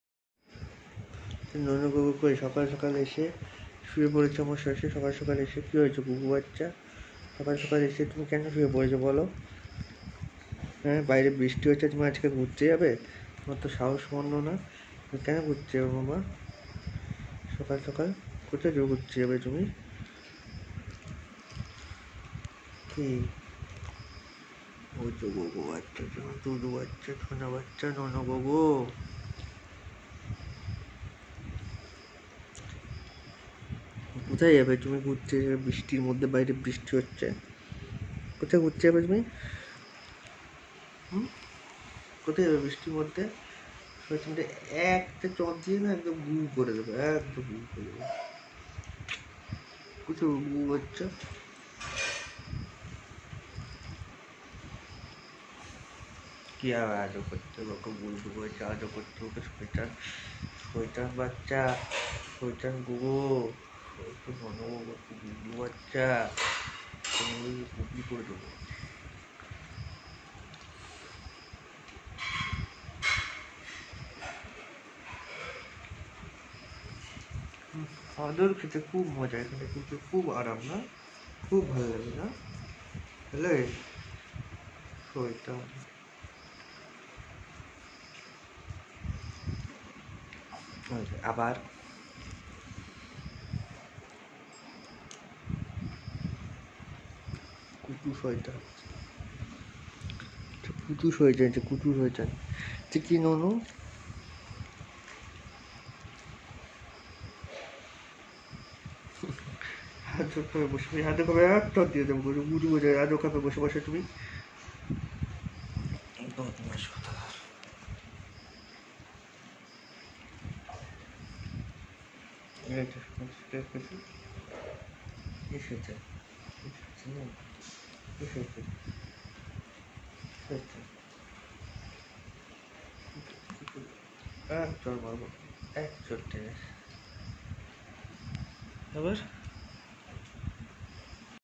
Morning love by Popins 🐕🦮🐕‍🦺
Morning interaction and sharing love with my pet.